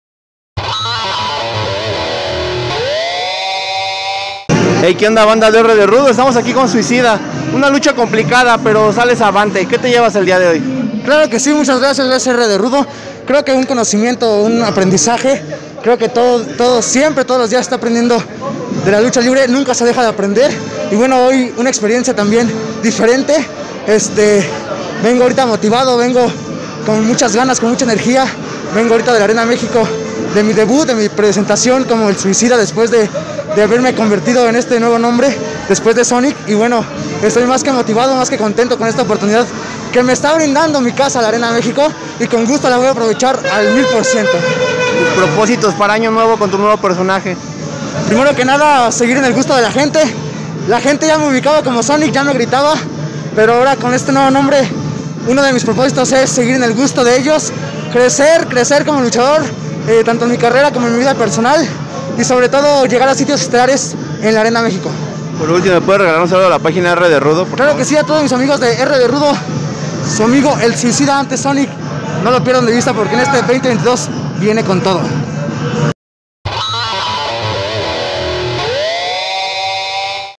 El Suicida: "Motivado por usar ahora este nombre"
El Suicida, antes Sonic en CMLL, está contento con su cambio de nombre y las retos que significa

Te acercamos a tus luchadores, en exclusiva. Obtén mercancía oficial y boletos. ÚNETE A a RDR Fan Access